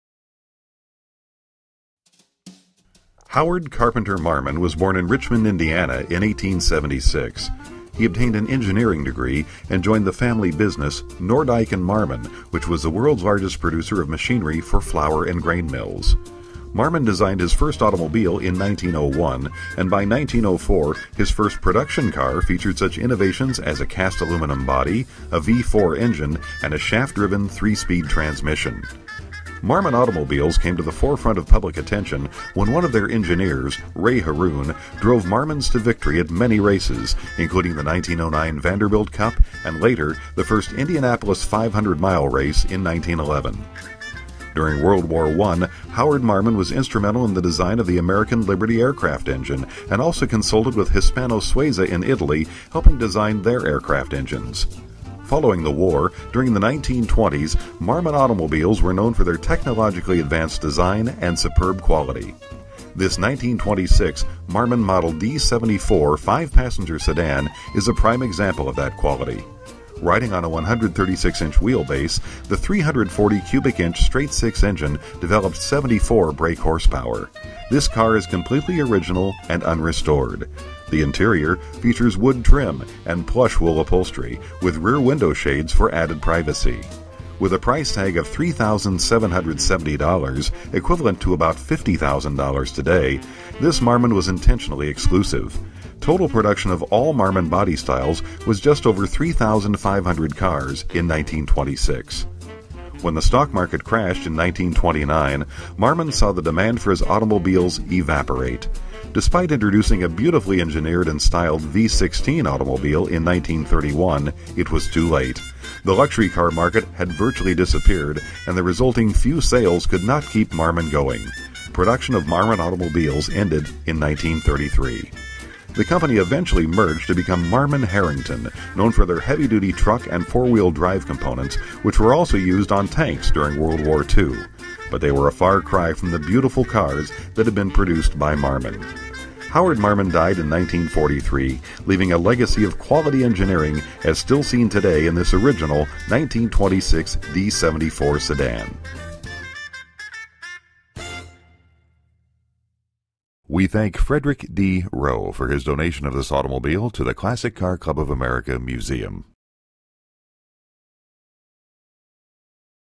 1926 Marmon D-74 Sedan. CCCA Museum
Displayed at the Classic Car Club of America Museum.